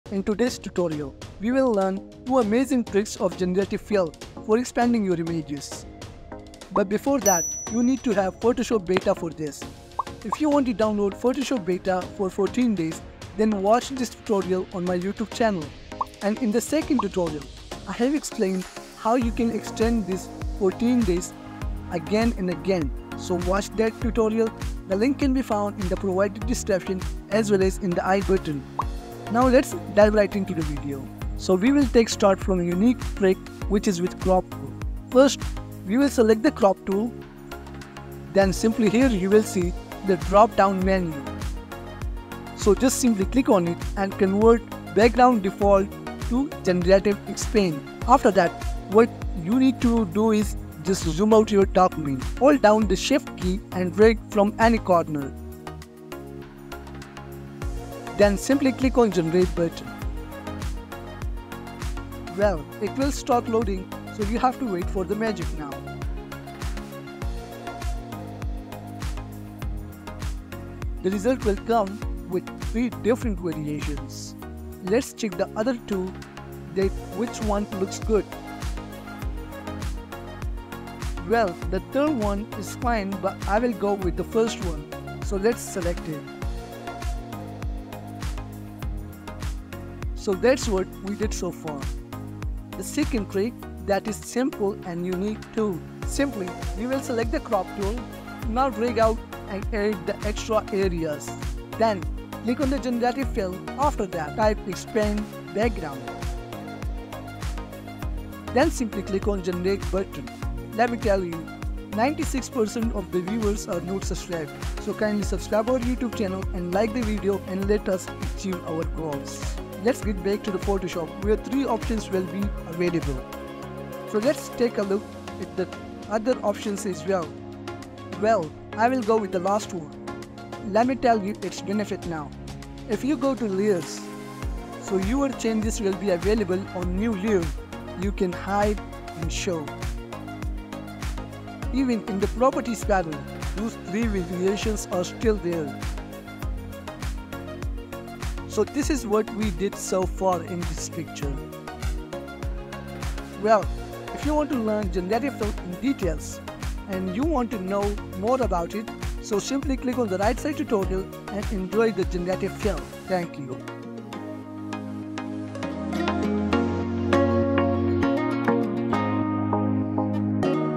Photoshop AI Magic: Expand Images with Ease! New Generative Fill
Assalam Alaikum:-
Photoshop AI Magic: Expand Images with Ease! New Generative Fill So PHOTOSHOP BETA has an exciting new AI Tool and that is GENERATIVE EXPAND. This new feature is found in the Crop Tool and it takes cropping to a whole new level. Join me in today's tutorial and lets check out GENERATIVE EXPAND...

► YouTube Chapters
0:00 Intro
0:30 A New Feature In The Crop Tool
1:32 expand image in photoshop ai
2:33 Outro

► Topics COVERED:

How to Use Generative Fill in Photoshop (beta) | Tutorial for Beginners | Adobe